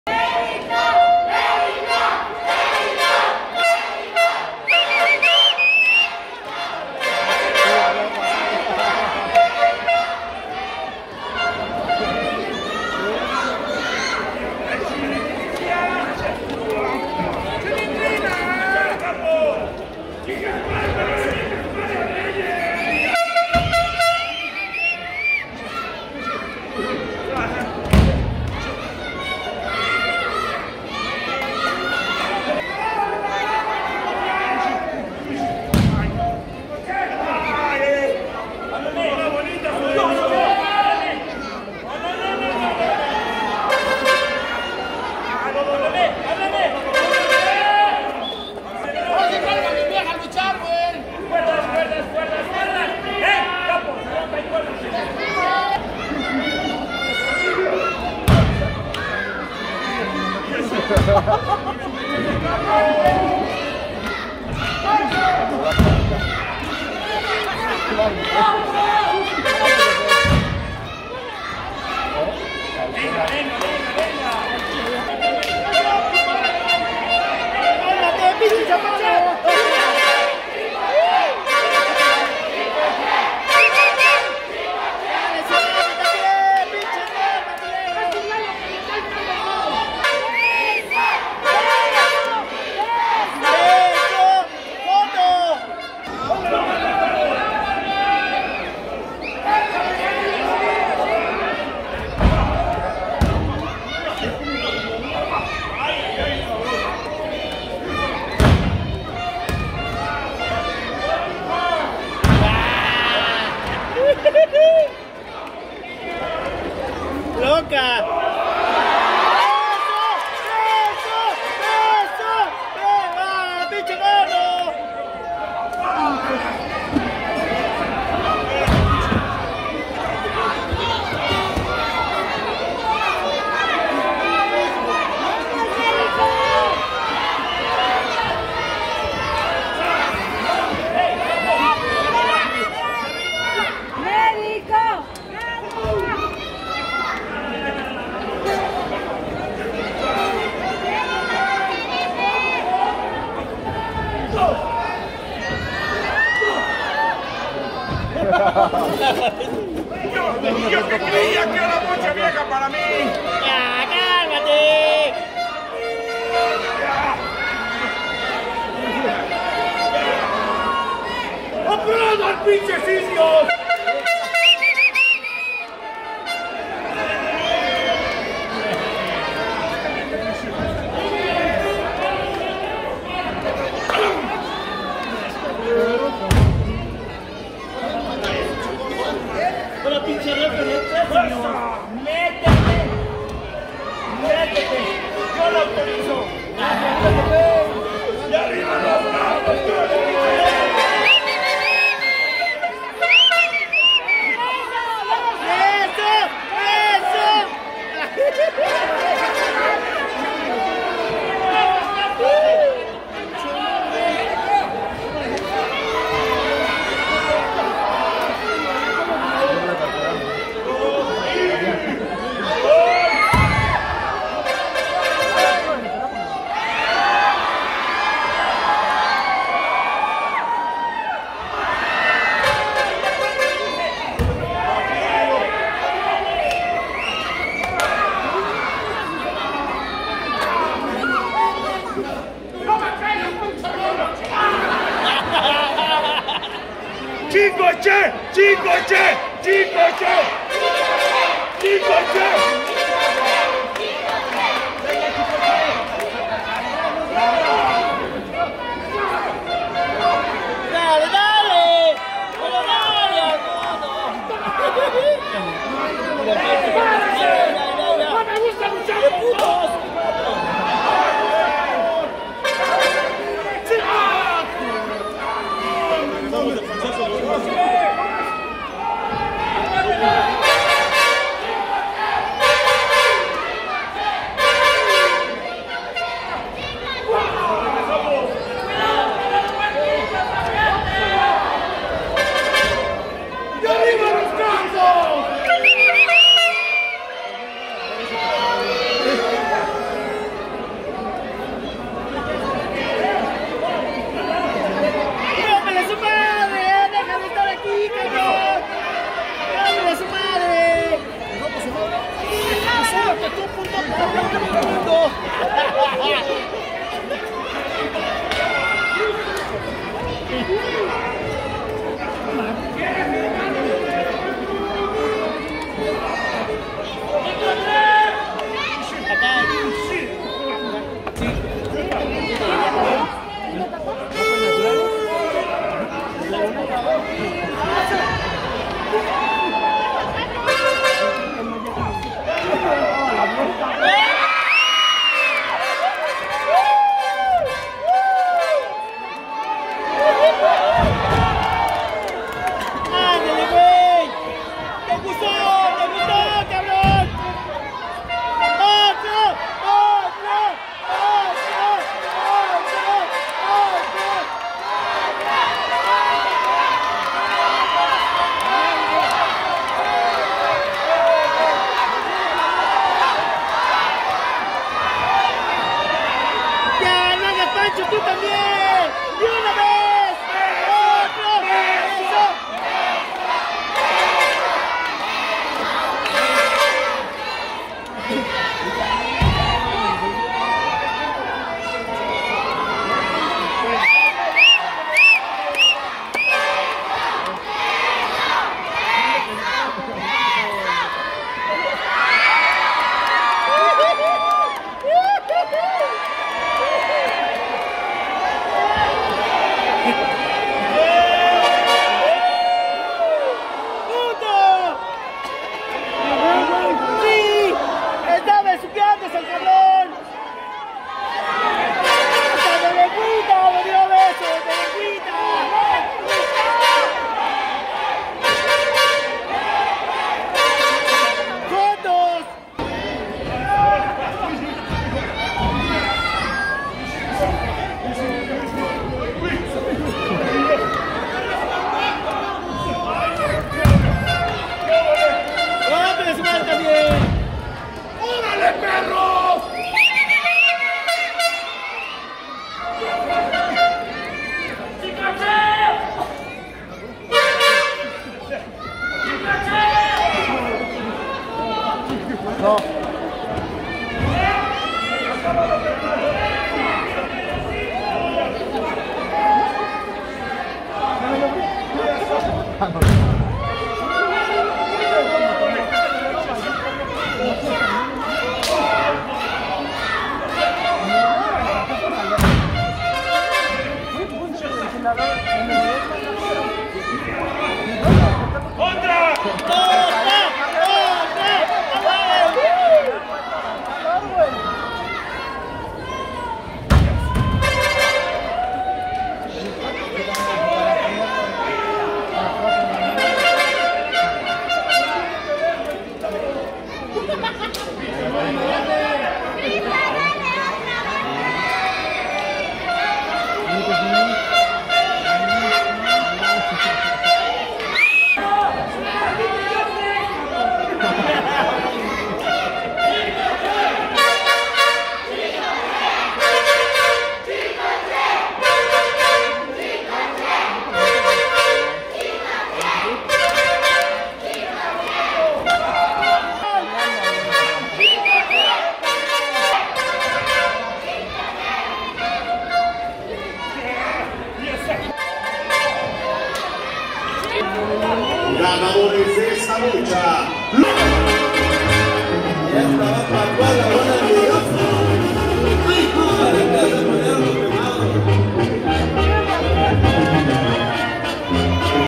LOS CAPOS Y OVETT CONTRA CHICO CHE, EL HIJO DEL MÉDICO ASESINO Y PASIÓN KRISTAL
DOMINGO 12 DE MAYO DEL 2019
ARENA NAUCALPAN